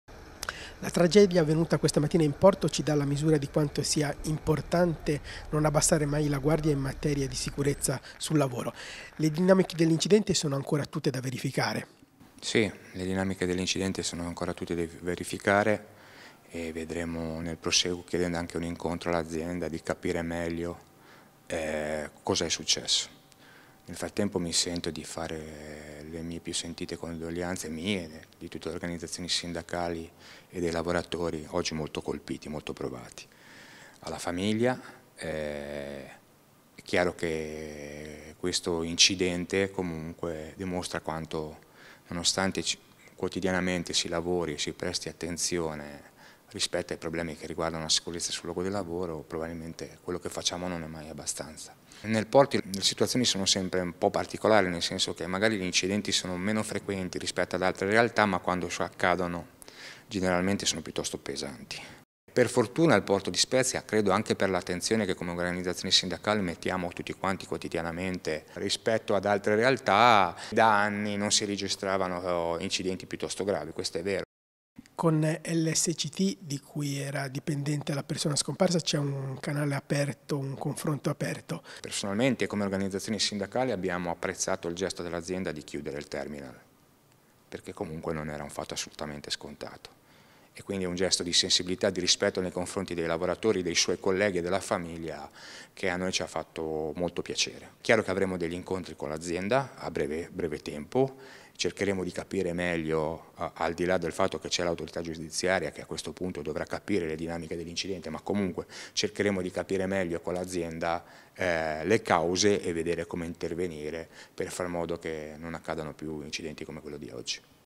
Morto in porto, la reazione del sindacato 15-12-2022
I sindacati esprimono le loro condoglianze alla famiglia del lavoratore scomparso e sottolineano quanto non si faccia mai abbastanza in materia di sicurezza sul lavoro. Con Lsct il confronto è aperto, apprezzato il gesto di chiudere il terminal.